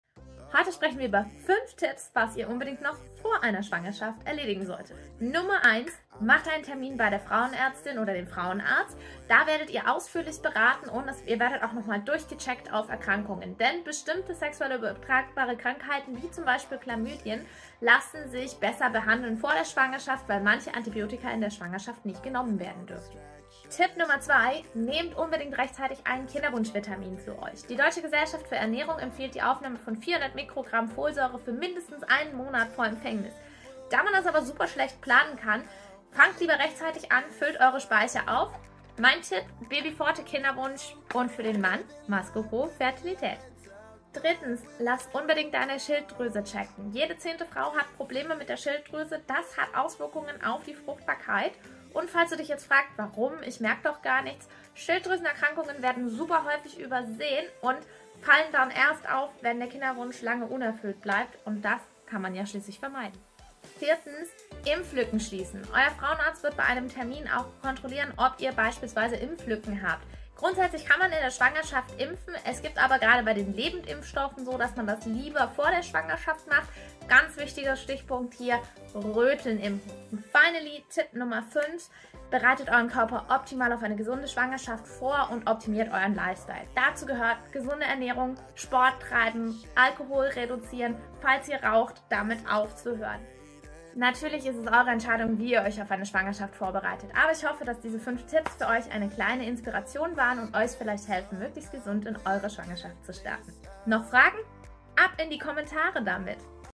Vorbereitung auf die Schwangerschaft: 5 Wertvolle Tipps für Paare im Kinderwunsch!💫
Die Vorbereitung auf die Schwangerschaft ist ein wichtiger Schritt auf dem Weg zum Elternglück. Sarah von @‌babybauchblog erläutert Euch wichtige Hinweise, die euch dabei unterstützen können. 👶💗